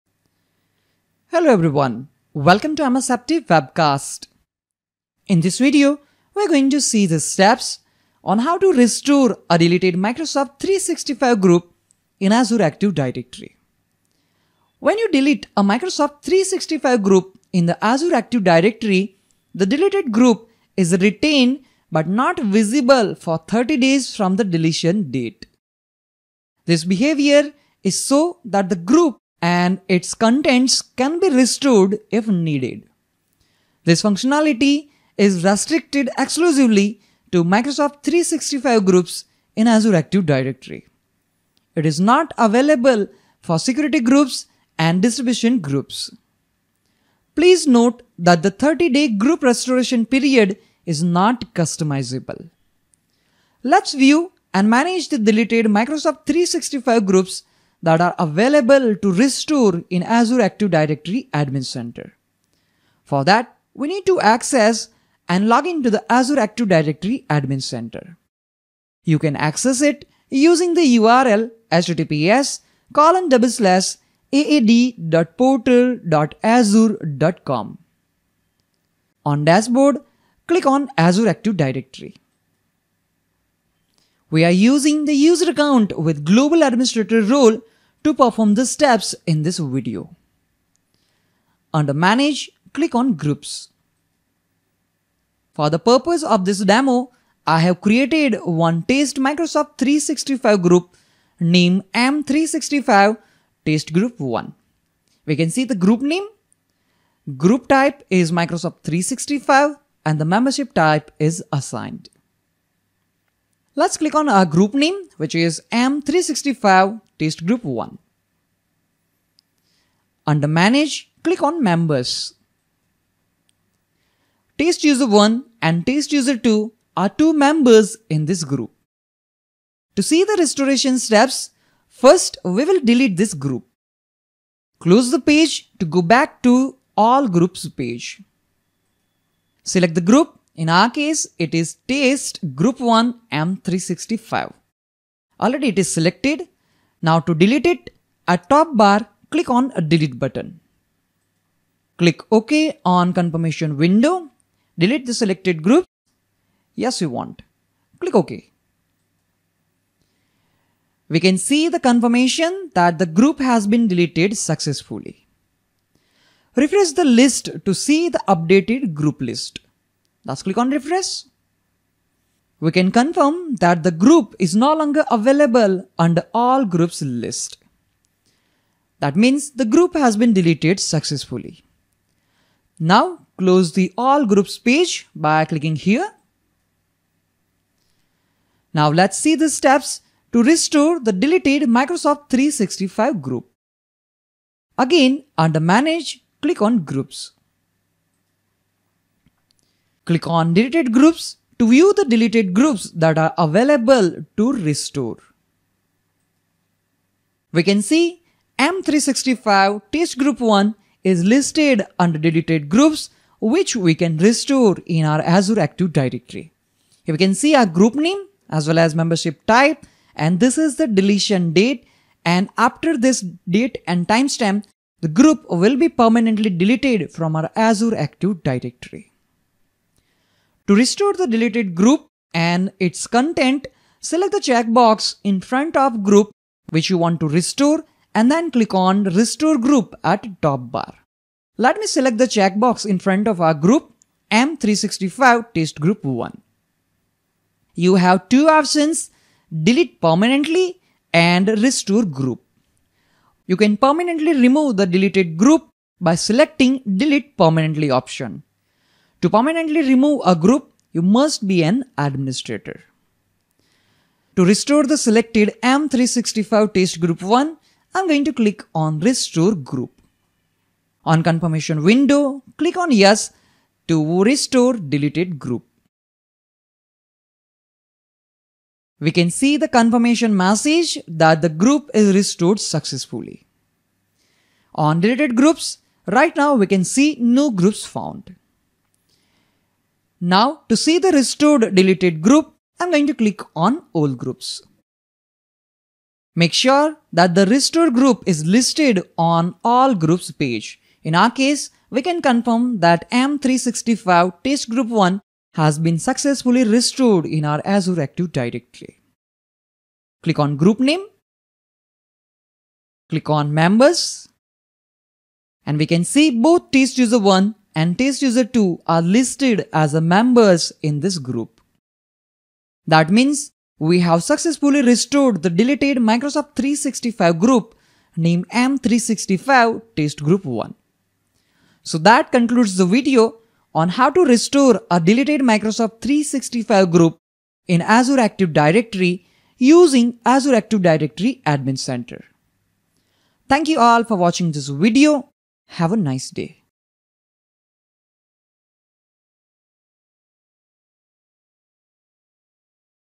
22. How to Restore a deleted Microsoft 365 group in Azure AD
Microsoft Azure Active Directory Beginners Video Tutorials Series:
This is a step by step guide on How to Restore a deleted Microsoft 365 group in Azure AD using Azure Active Directory Admin Center web interface.